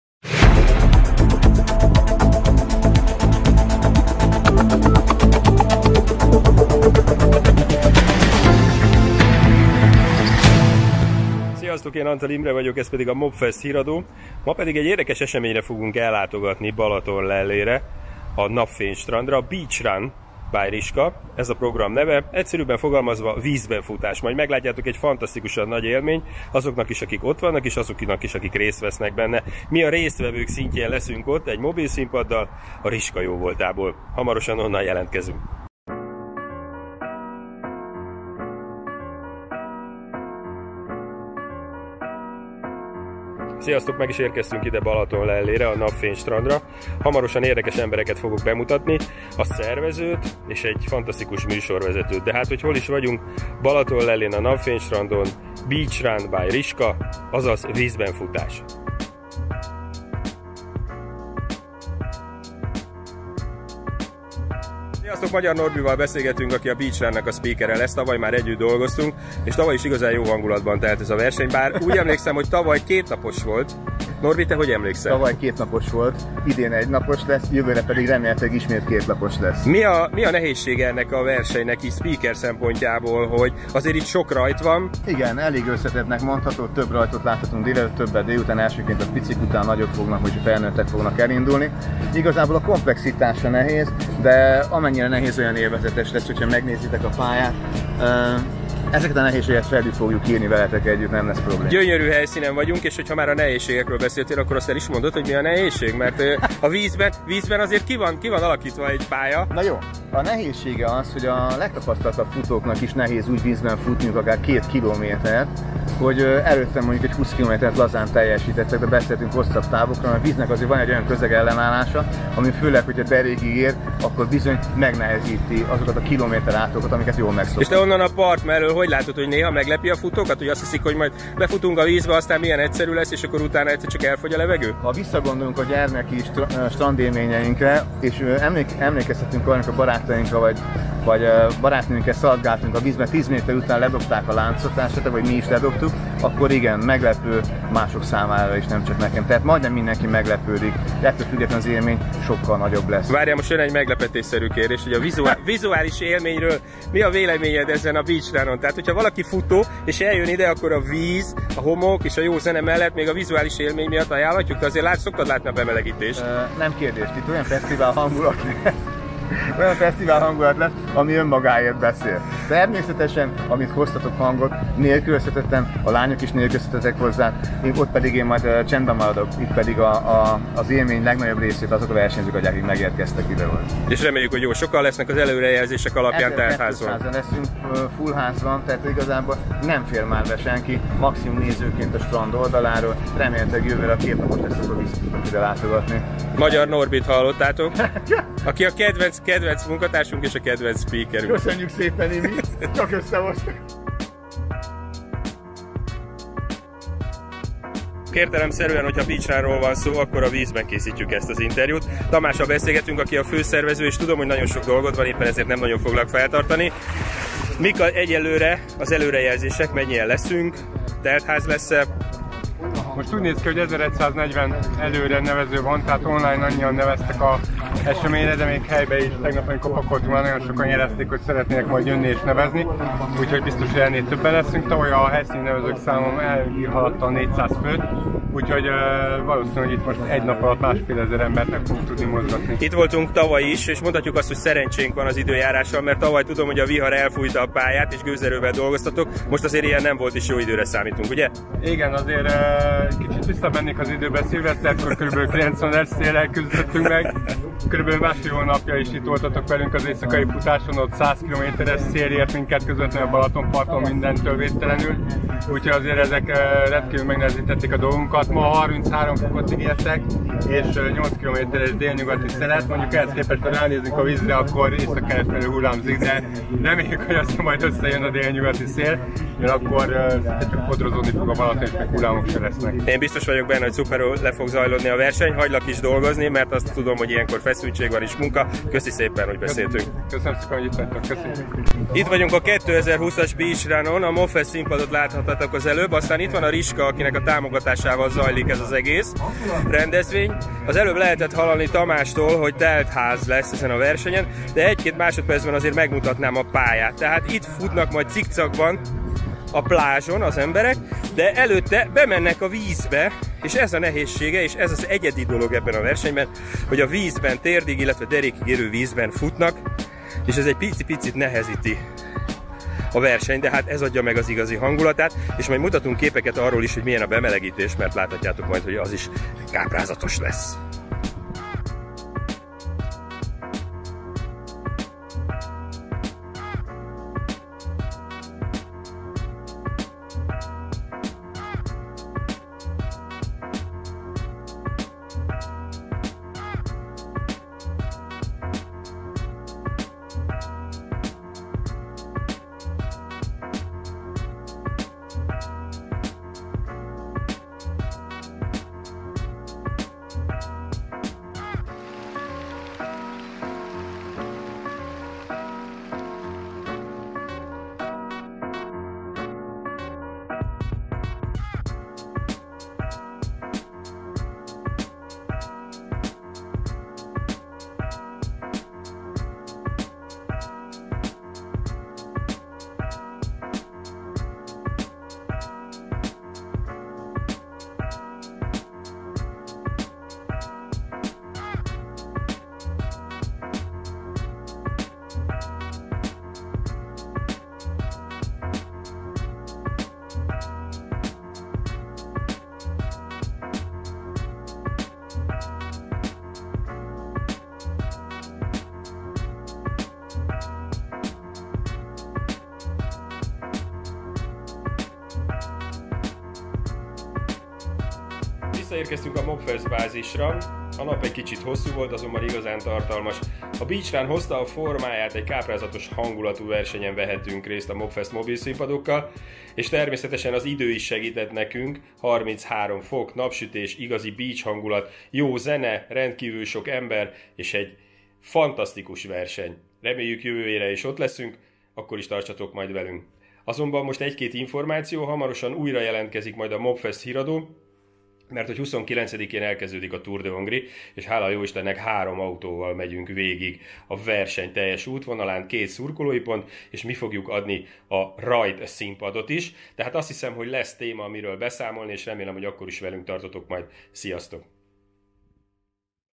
Mobfeszt Híradó_Beach Run by RISKA Balatonlelle
Beach Run by Riska
Vízben futás Balatonlellén
Színpadot adunk vágyaidnak!